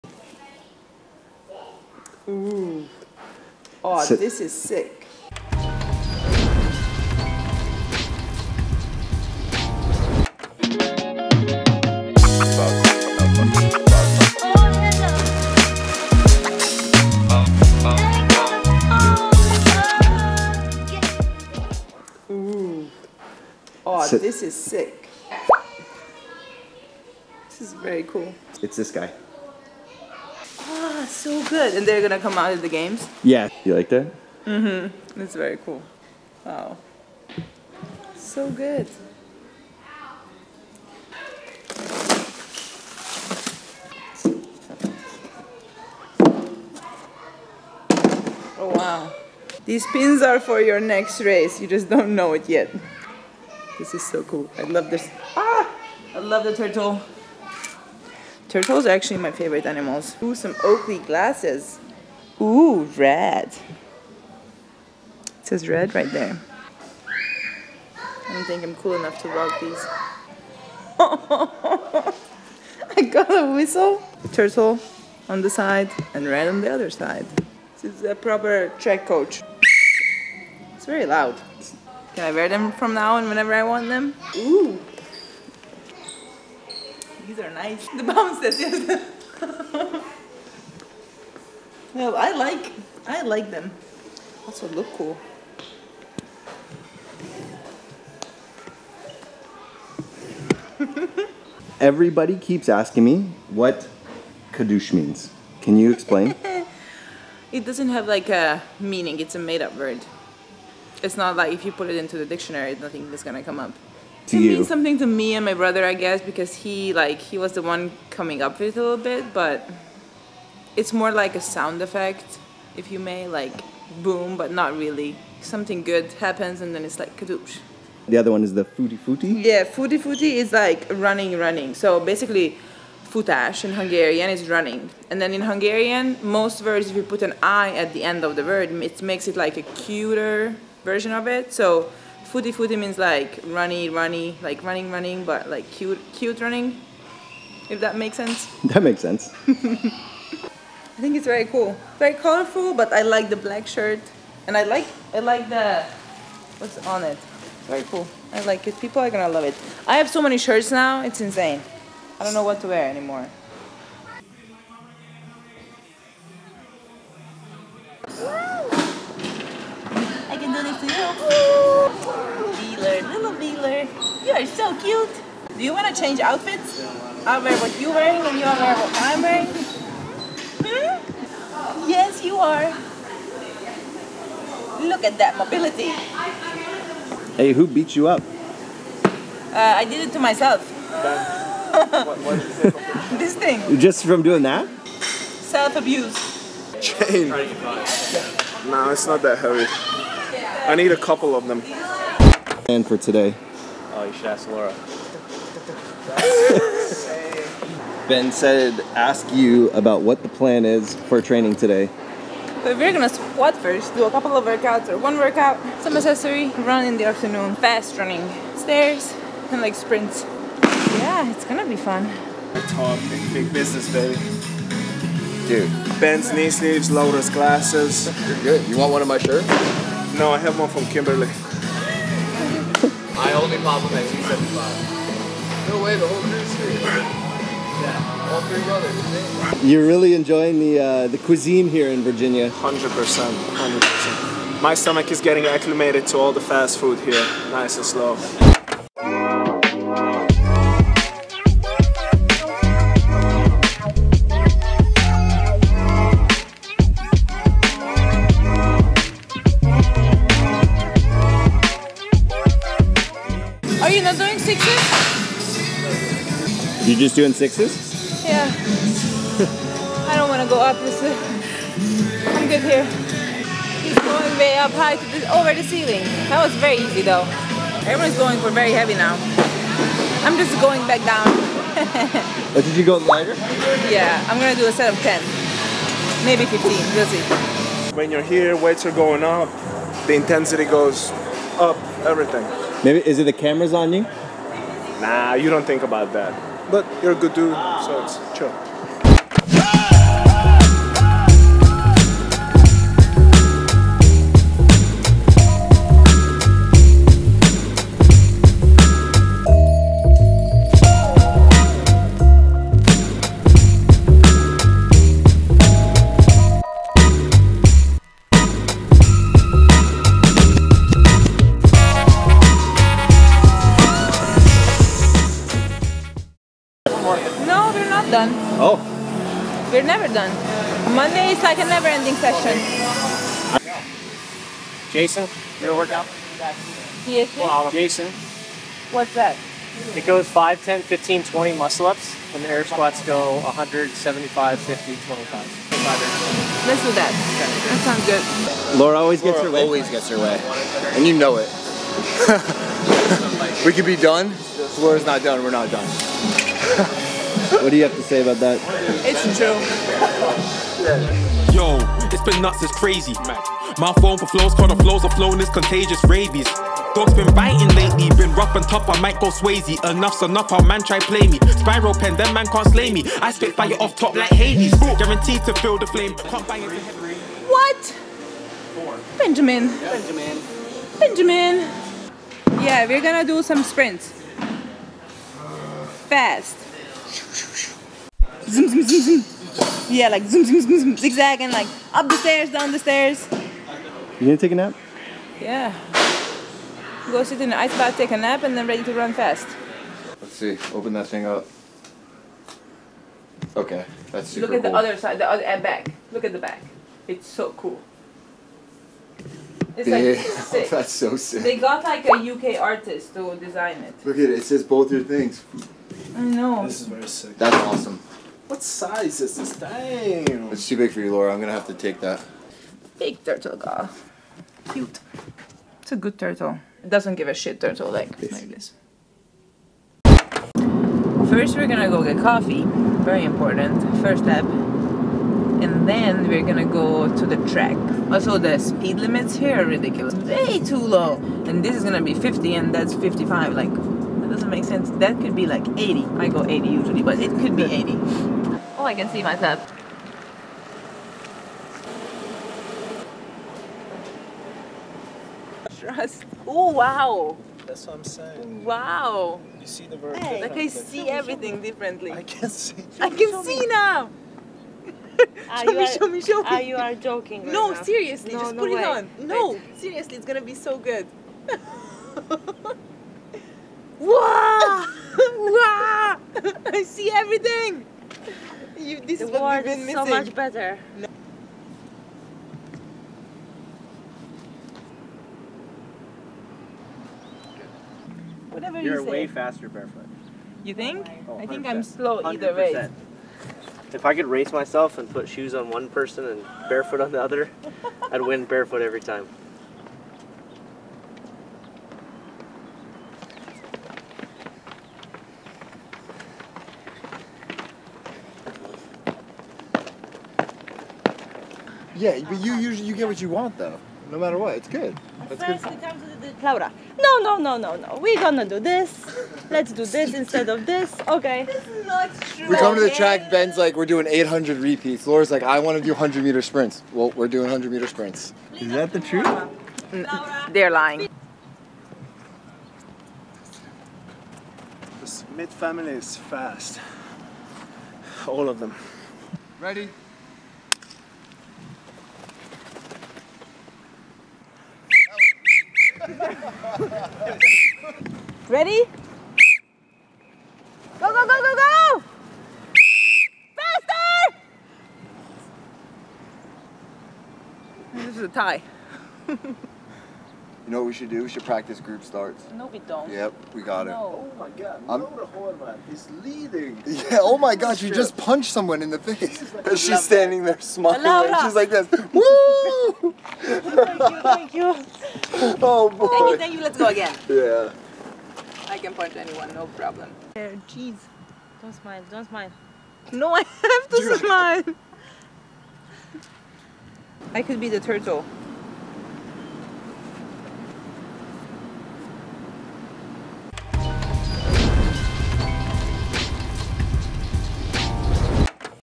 Laura Horvath CrossFit Games Camp - R.A.D® Tapes - Episode 21
In this latest episode of R.A.D® Tapes, we head to Virginia to see Laura Horvath. We catch up with her and her crew as they prepare for the 2023 Crossfit Games. We also brought some surprise gifts with us.